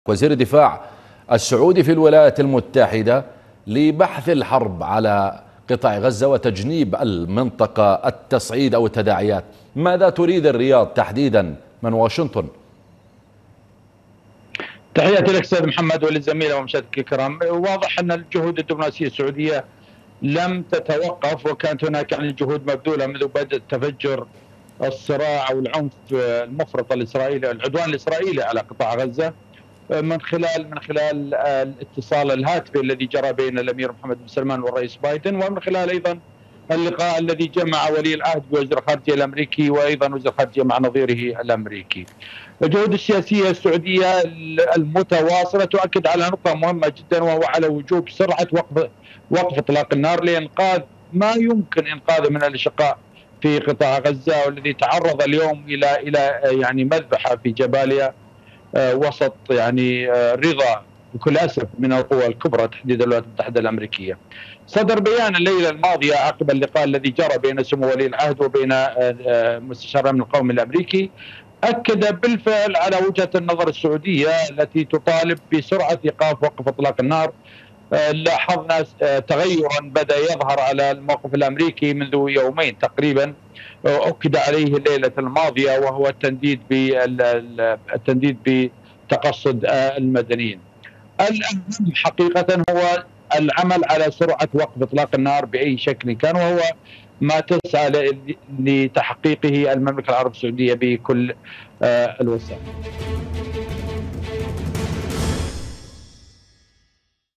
شاهد .. تحركات سعودية عاجلة لوقف الحرب الاسرائيلية على غزة
موقع قناة التغيير الفضائية  الالكتروني